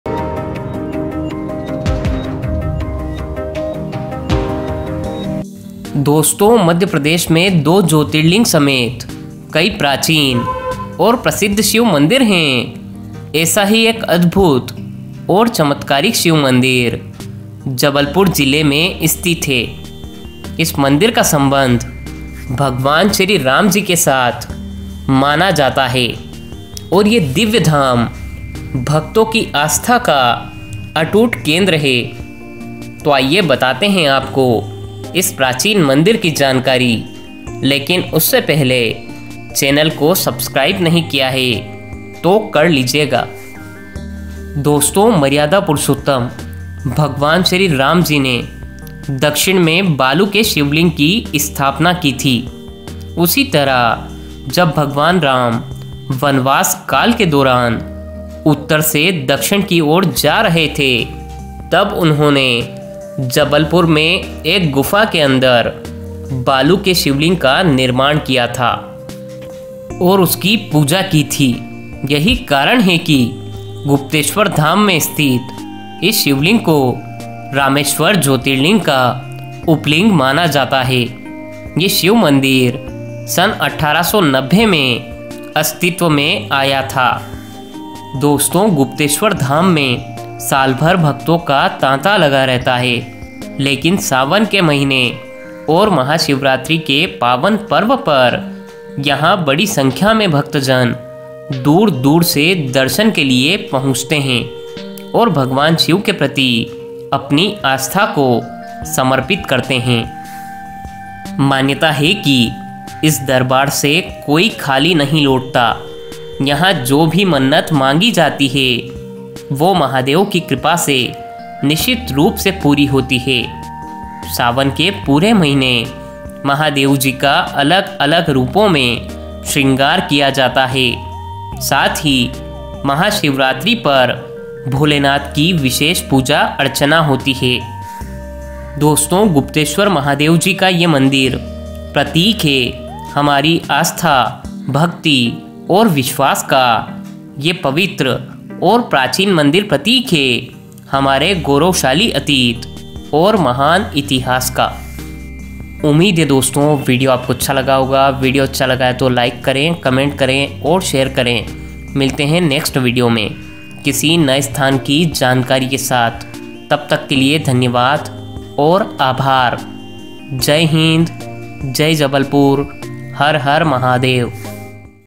स्वयं भगवान श्री राम ने की थी यहाँ शिवलिंग की स्थापना | Gupteshwar Mahadev Jabalpur Madhyapradesh
स्वयं भगवान श्री राम ने की थी यहाँ शिवलिंग की स्थापना गुप्तेश्वर महादेव मंदिर जबलपुर मध्यप्रदेश  Gupteshwar Mahadev Jabalpur Madhyapradesh
नमस्कार दोस्तों आज के विडियो में जानिए मध्यप्रदेश के जबलपुर में स्थित गुप्तेश्वर महादेव मंदिर की जानकारी यहाँ पर स्वयं भगवान श्री राम जी ने शिवलिंग की स्थापना की थी |
Dosto Jabalpur me sthit gupteshwar mahadev mandir ko rameshwar jyotirling ka upling mana jata hai.
Create by – mera shining india
Manish Sharma
Credit of photos – google,local people .
Jai hind jai Jabalpur har har mahadev